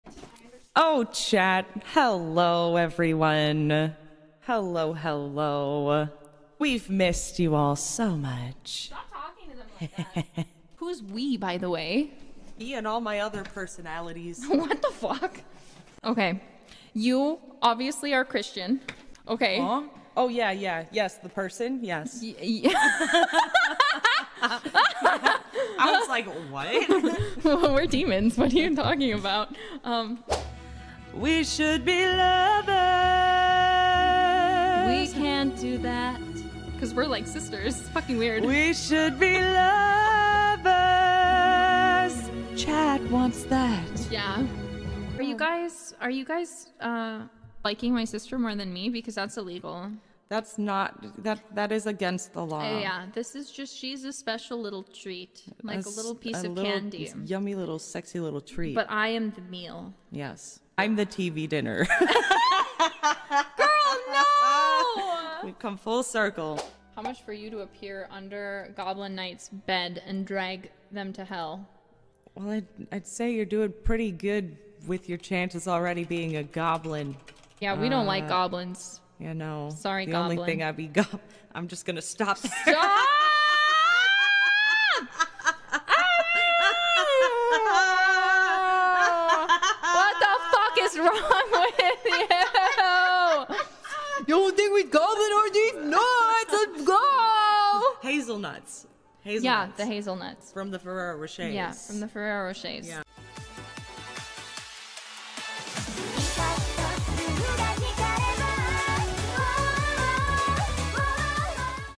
𝗔𝗿𝗮𝗱𝗶𝗮 𝗷𝘂𝘀𝘁 𝗯𝗲𝗶𝗻𝗴 𝗮𝗻 𝗮𝗯𝘀𝗼𝗹𝘂𝘁𝗲 𝗵𝗲𝗮𝗱𝗮𝗰𝗵𝗲 𝗳𝗼𝗿 𝗡𝗲𝗿𝗶𝘀𝘀𝗮 |Hololive
【UNARCHIVED KARAOKE】Singing some love songs~ 🎼

Hololive
Hololive ch
hololive reaction
hololive animation
hololive english
holoadvent
hololive advent
Holocure

ED ART by @Tkminnm from Twitter -

BGM used
DOVA-SYNDROME